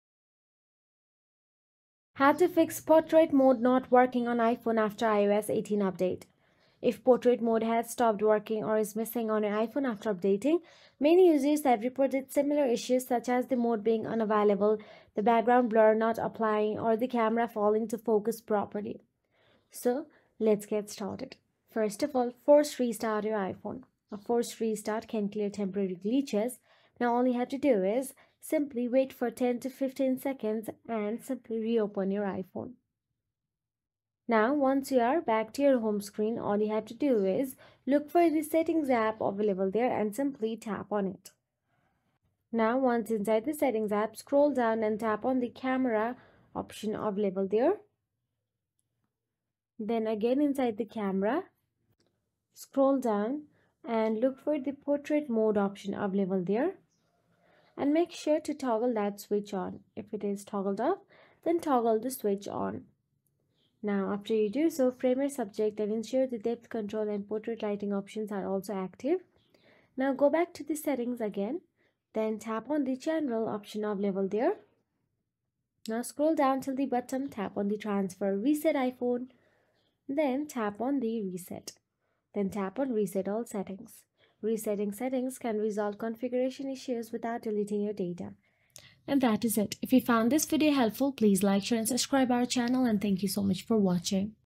How to Fix Portrait Mode Not Working on iPhone After iOS 18 Update
Welcome to iPhone tips,

In this video you will learn about How to Fix Portrait Mode Not Working on iPhone After iOS 18 Update.

How to Fix Portrait Mode Not Working on iPhone After iOS 18 Update (2025)!
Is Portrait Mode not working on your iPhone after updating to iOS 18? This video will guide you through the steps on how to fix Portrait Mode issues on your iPhone quickly and easily. Whether you're asking, "Why is Portrait Mode not working after iOS 18 update?" or need help troubleshooting camera problems, this tutorial has you covered. Learn how to restore camera features, adjust settings, and get Portrait Mode working again. Watch now to see how you can fix Portrait Mode on iPhone after iOS 18 today!

Timestamps:

00:00 How to Fix Portrait Mode Not Working on iPhone After iOS 18 Update
00:10 Why Is Portrait Mode Not Working on iPhone After iOS 18?